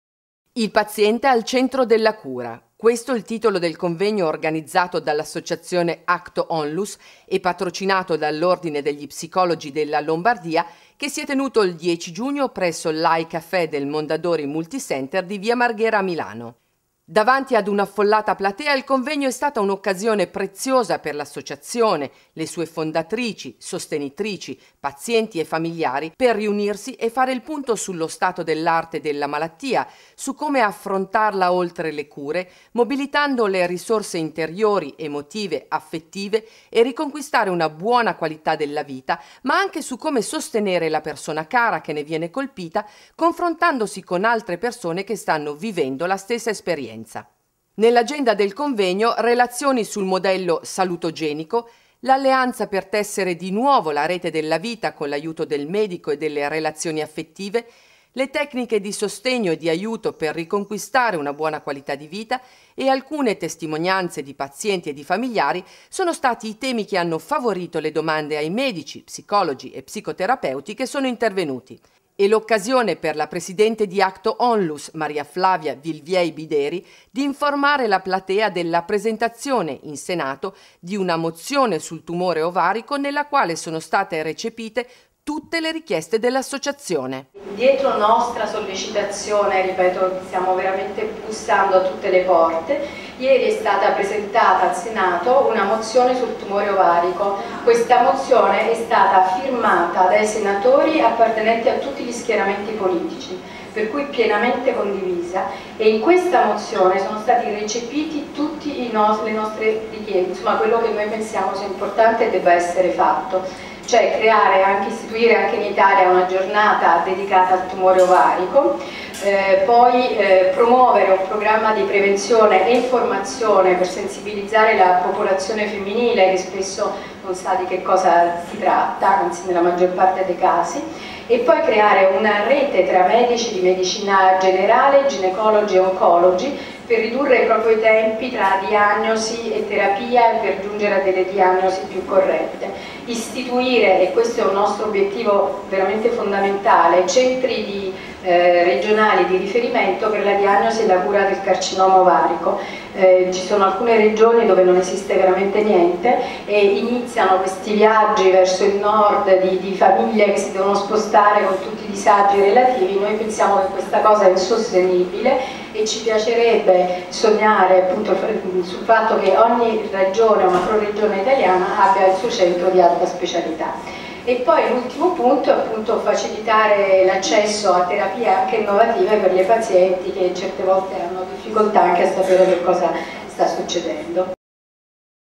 Acto presenta "Il Paziente al centro della Cura
HI Caffè Mondadori - Via Marghera n. 28, Milano
venerdì 10 giugno 2011 dalle 16:30 alle 20:00

Come affrontare la malattia? Come sostenere una persona cara che ne è stata colpita?
Si parlerà di come spostare l'attenzione dalla malattia alla parte sana del paziente per mobilitare le risorse interiori, emotive, affettive e riconquistare una qualità della vita.
Sarà un'ottima occasione per rivolgere domande a medici, psicologi, psicoterapeuti e per confrontarsi  con altre persone che stanno vivendo la stessa esperienza.